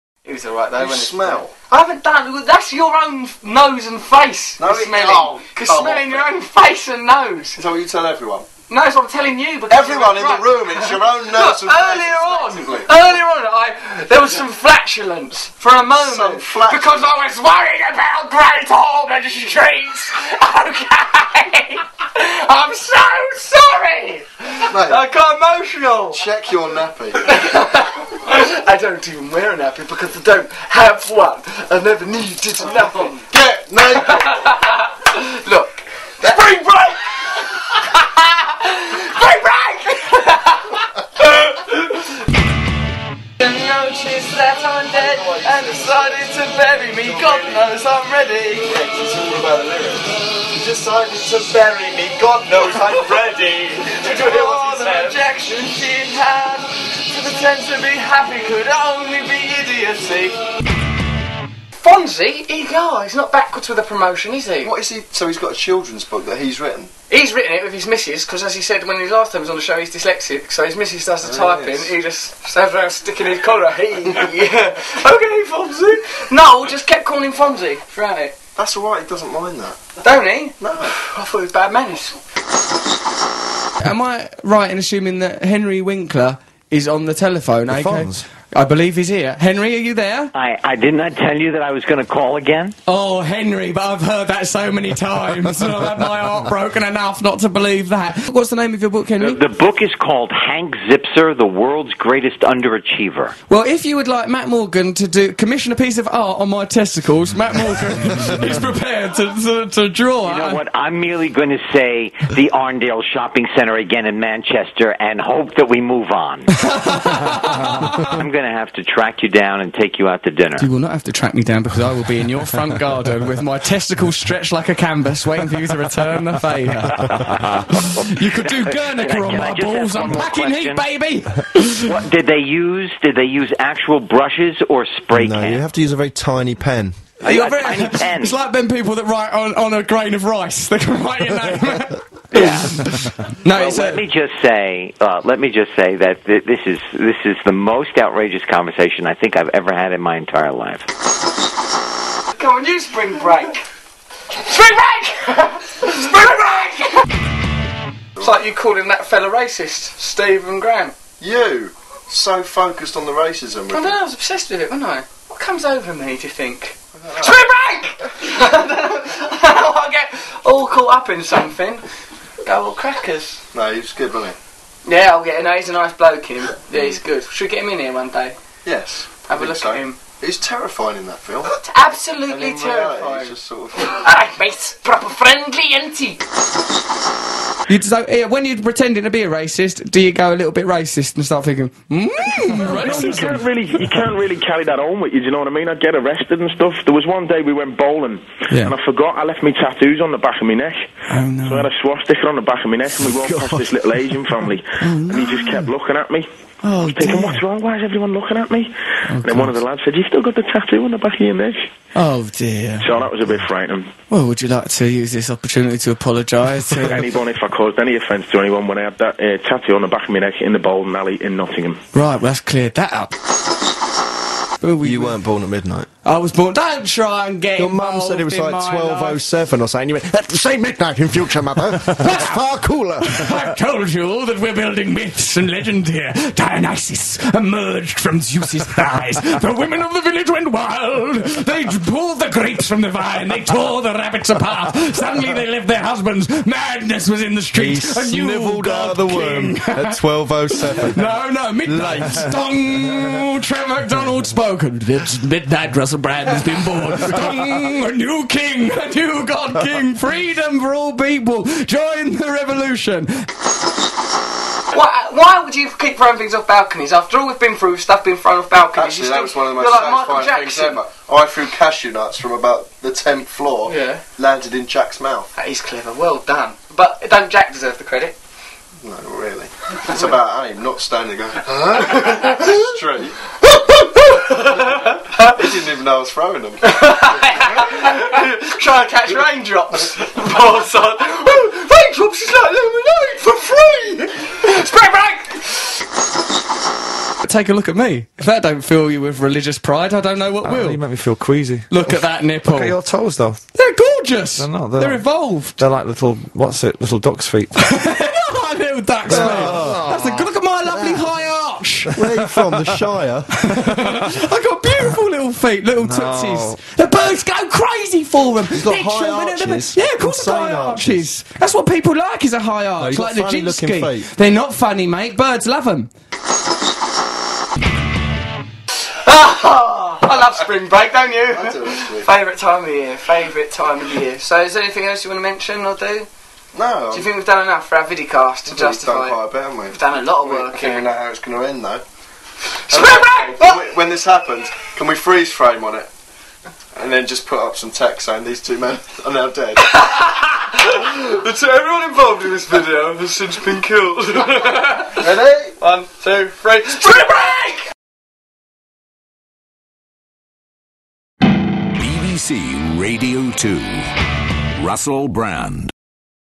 Russell Brand and Matt Morgan Viddycast #4 Spring Break
Includes Henry Winkler, Noel Gallagher and a lot of flashing from the boys.

!Spring Break!